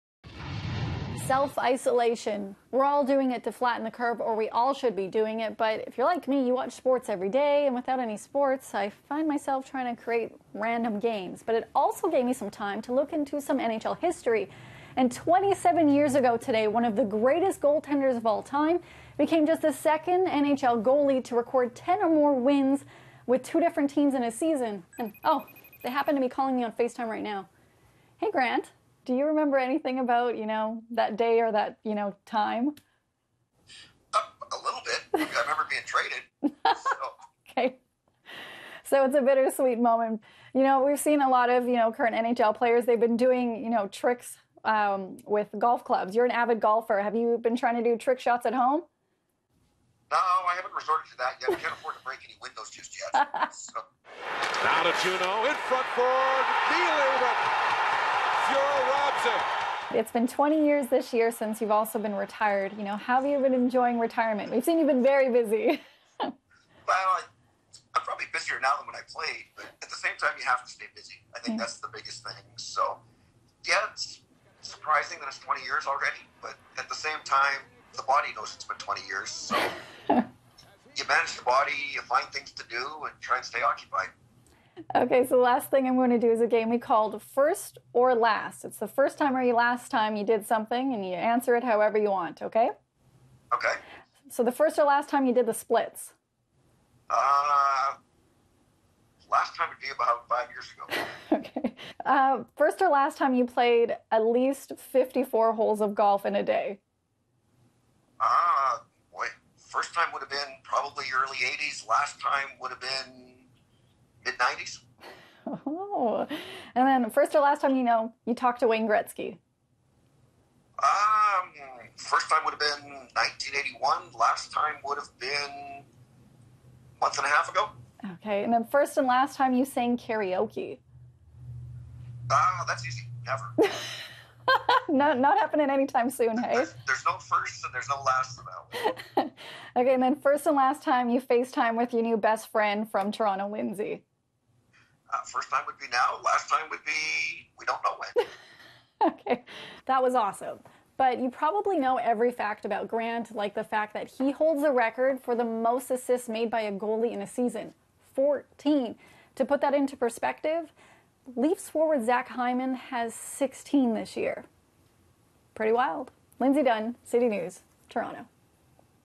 NHL legend remembers moment in hockey history
Remembering a hockey moment in history with NHL legend Grant Fuhr. And what does a hockey legend do when there is no hockey to watch on TV? Lindsay Dunn has the story.